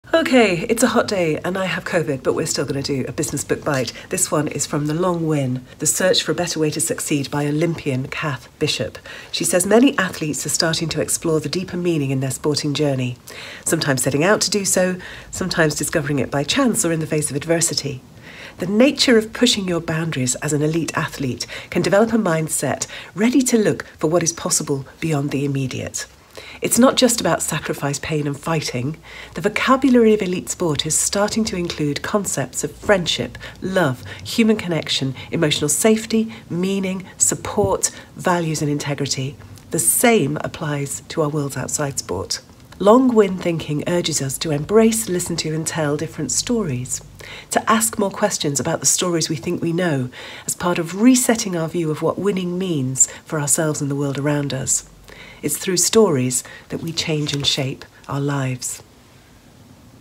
businessbookbites No.15: The Long Win by Cath Bishop
Okay, it's a hot day and I have COVID, but we're still good for a businessbookbite. This one is from The Long Win: The search for a better way to succeed, by Olympian Cath Bishop. She says

'Many athletes are starting to explore the deeper meaning in their sporting journey: sometimes setting out to do so, sometimes discovering it by chance or in the face of adversity. The nature of pushing your boundaries as an elite athlete can develop a mindset ready to look for what is possible beyond the immediate. It's not just about sacrifice, pain and fighting. The vocabulary of elite sport is starting to include concepts of friendship, love, human connection, emotional safety, meaning, support, values and integrity.
The same applies to our worlds outside sport.

Long win thinking urges us to embrace, listen to, and tell different stories... to ask more questions about the stories we think we know as part of resetting our view of what winning means for ourselves and the world around us. It is through stories that we change and shape our lives.'